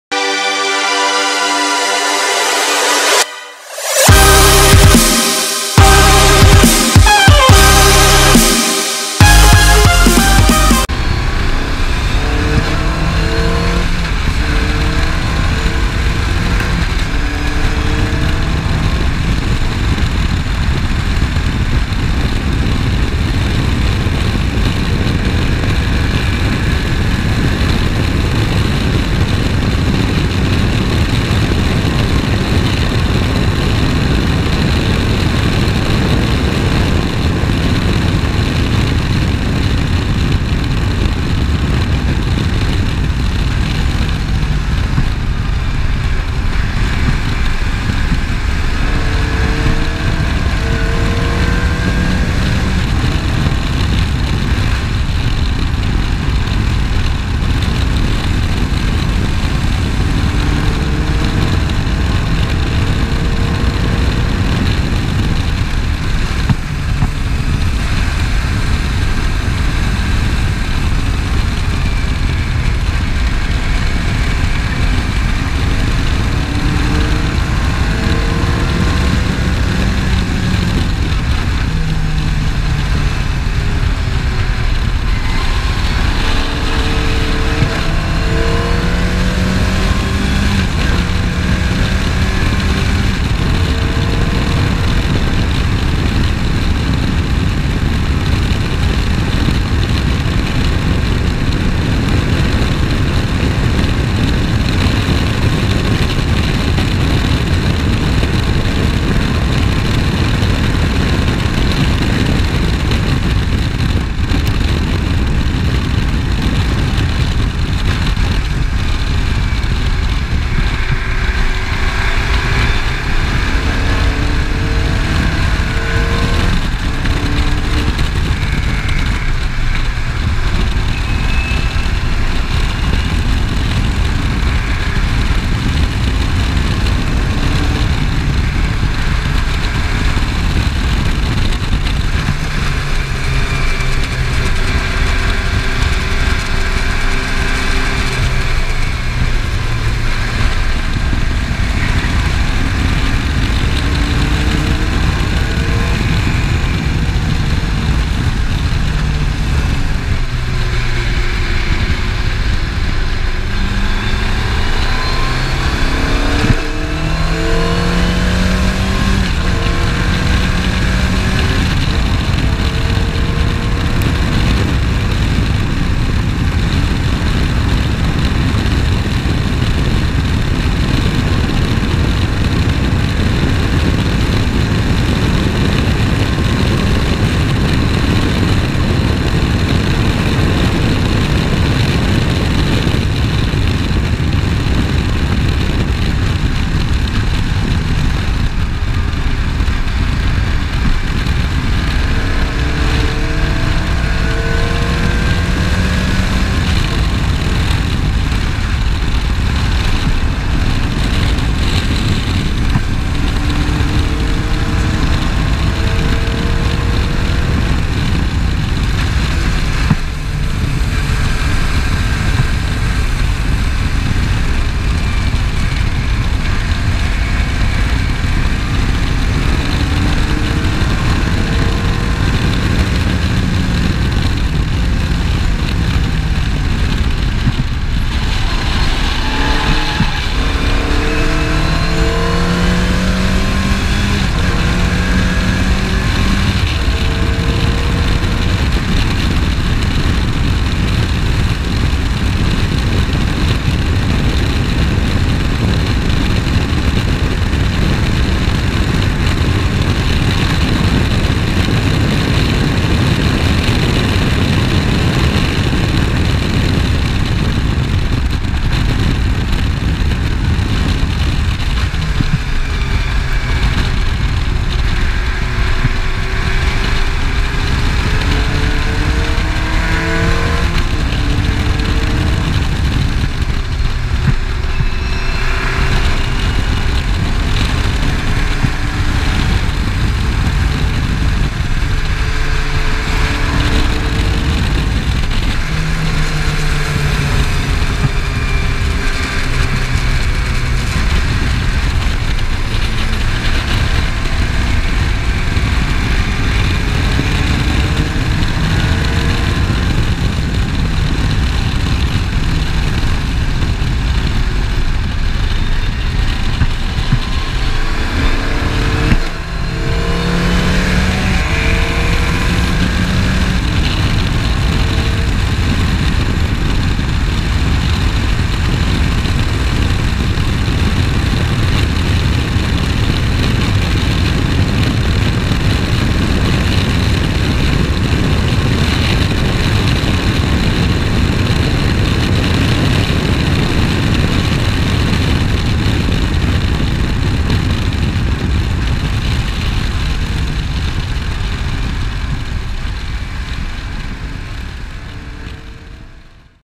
Skip Barber hot laps in the Formula Car!
This past weekend I got to attend the Skip Barber 3 day racing school. Needless to say. The 240 may be no more.